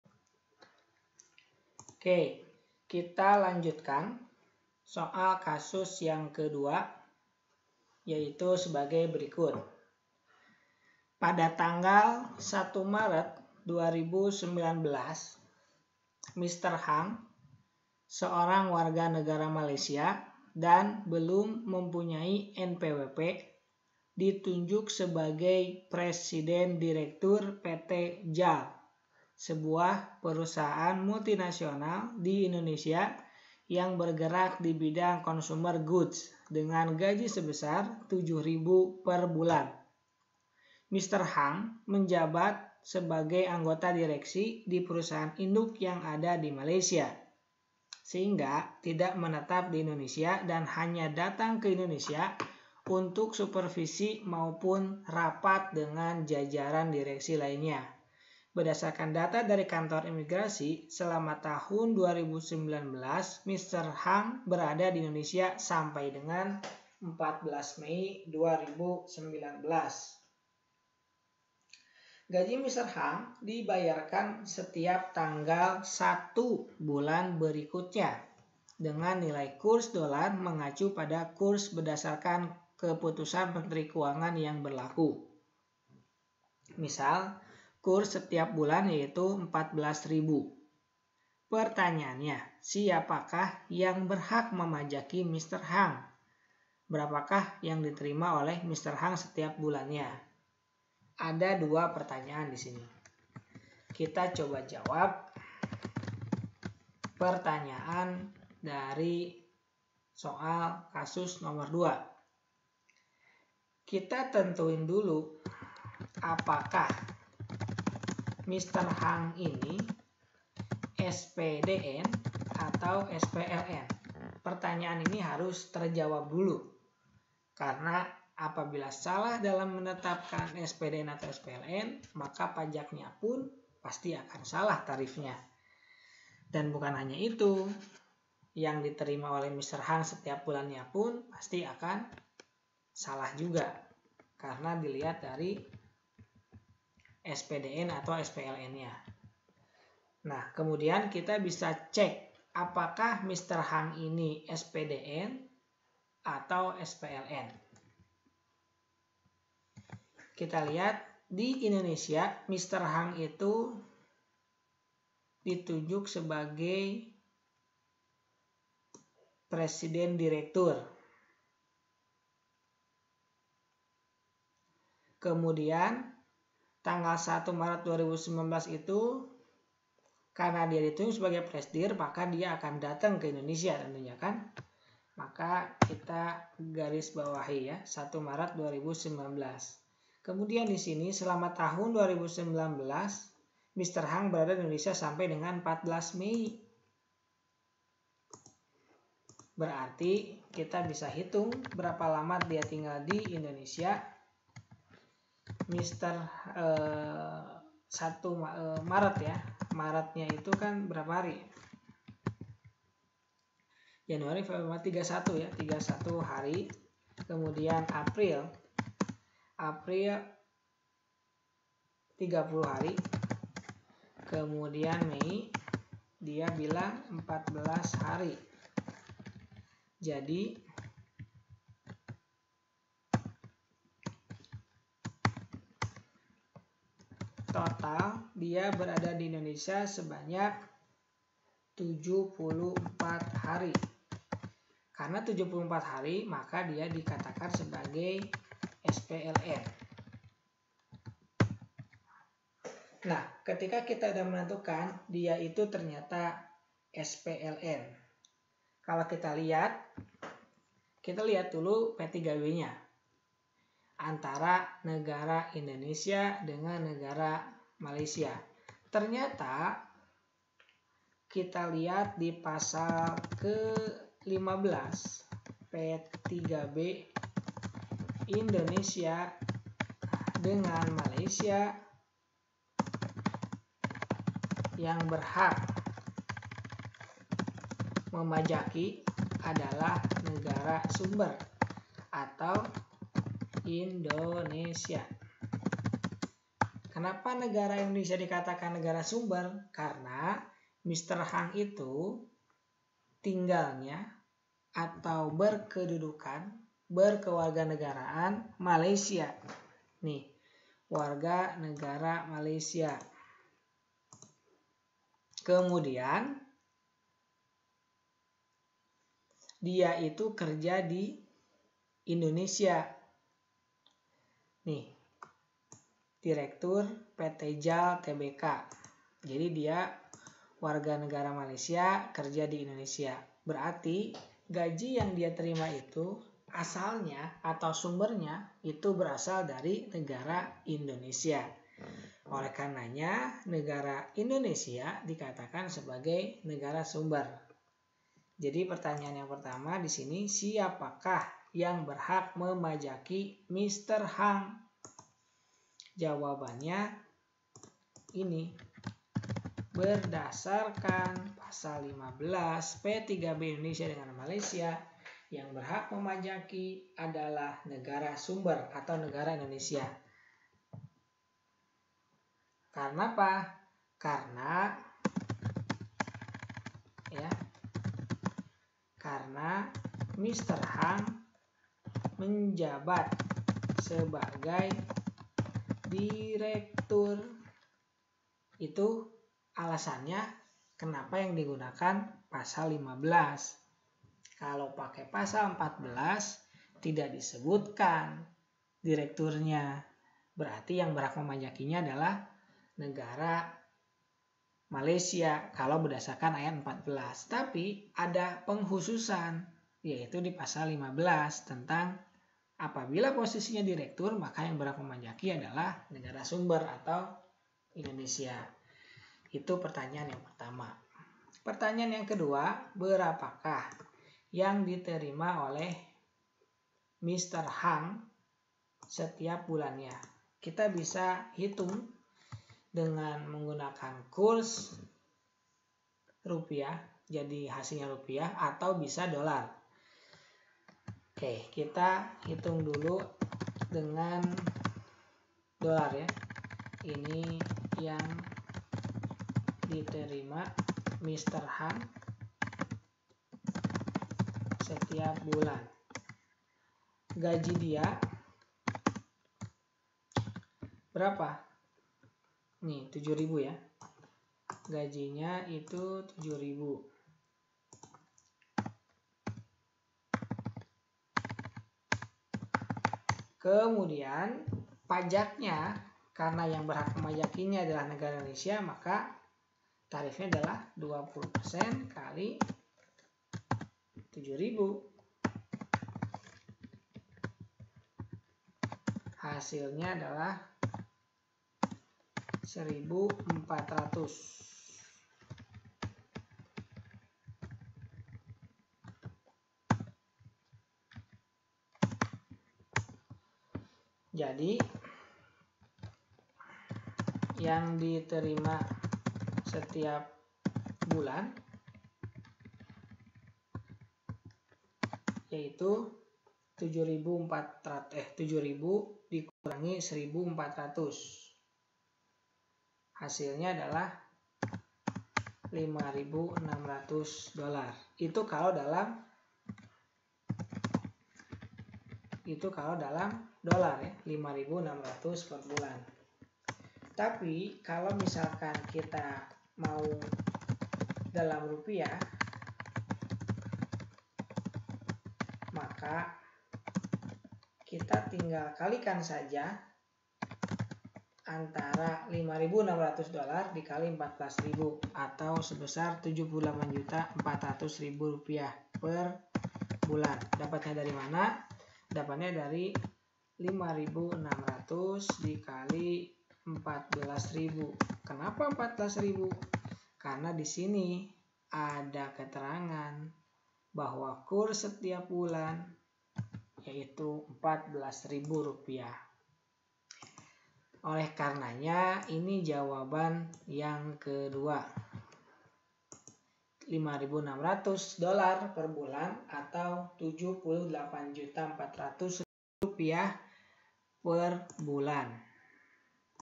Soal Kasus Pajak Internasional nomor 2
Pembahasan soal kasus pajak internasional yang diberikan dalam bentuk kuis yang telah dilakukan.